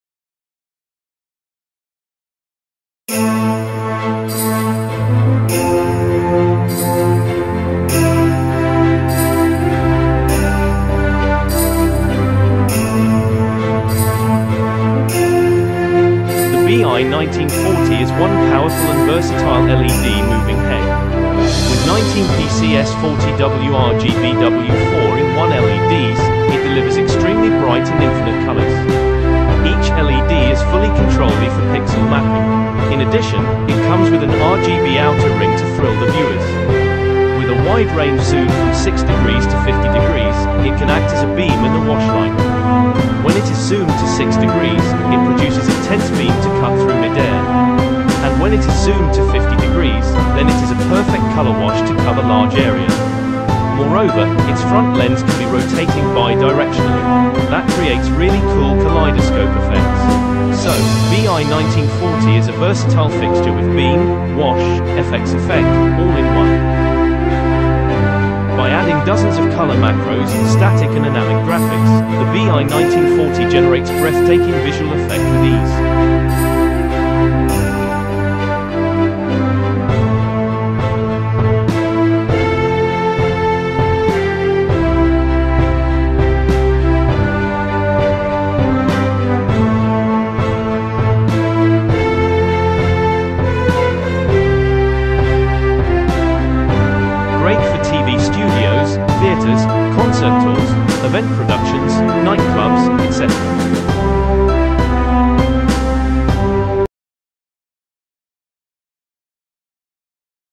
Bee Eye LED moving head 1940 19x40W RGBW 4in1 B-eye Beam Wash FX
The B-eye/Bee Eye 1940 is one powerful and versatile LED moving head. With 19pcs 40W RGBW 4-in-1 LEDs, it delivers extremely bright and infinite colors. Each LED is fully controlly for pixel mapping. In addition, it comes with an RGB outer ring to thrill the viewers.

With a wide range zoom from 6° to 50°, it can act as a beam and a wash light. When it is zoomed to 6°, it produces intense beam to cut through mid-air. And when it is zoomed to 50°, then it is a perfect color wash to cover large area. Moreover, its front lens can be rotating bi-directionally. That creates really cool kaleidoscope effects. So, B-eye/Bee Eye 1940 is a versatile fixture with beam, wash, FX effect, all in one. By adding dozens of color macros, static and ynamic graphics, the B-eye/Bee Eye 1940 generates breathtaking visual effect with ease.

Greate for TV studios, theatres, concert tours, event productions, night clubs, etc.